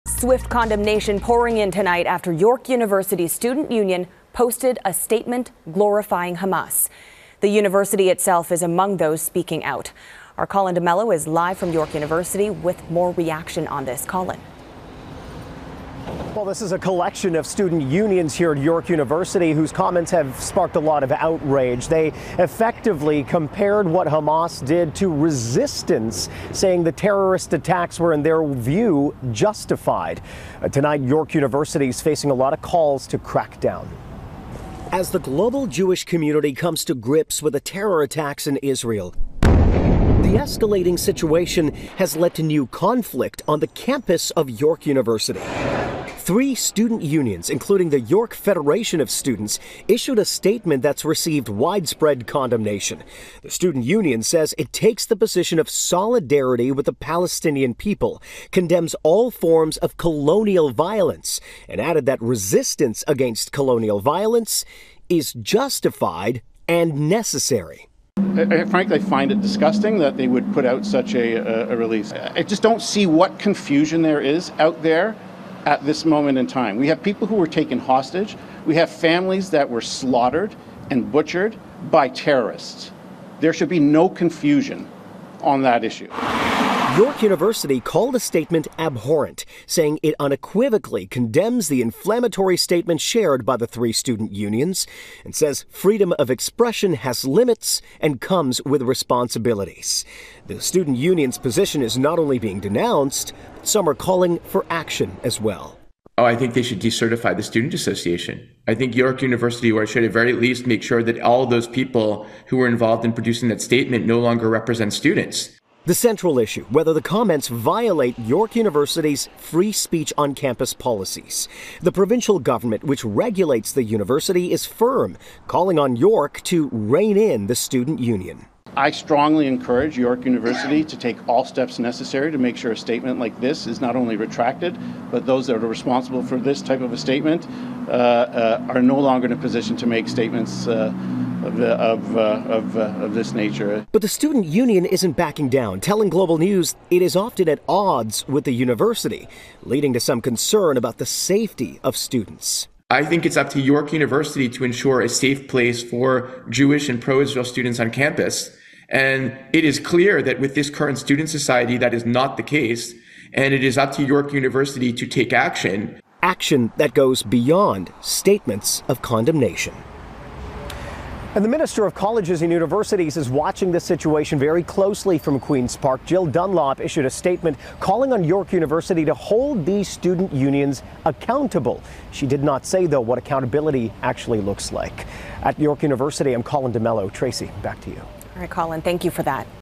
Israel-Gaza: University student union calls Hamas attacks "justified and necessary"
Toronto's York University is facing calls to decertify a major student union over a public statement that called Hamas’ attacks in Israel “justified and necessary.”

The York Federation of Students, which represents 55,000 undergraduate students at York University, issued a “statement of solidarity with Palestine” on Thursday, in which the union argued the actions of Hamas represented a “strong act of resistance.”

The statement, which was also signed by the York University Graduate Students Association, was met with condemnation from Liberal MP Anthony Housefather, who called the comments “shocking.”

Global's Colin D'Mello reports.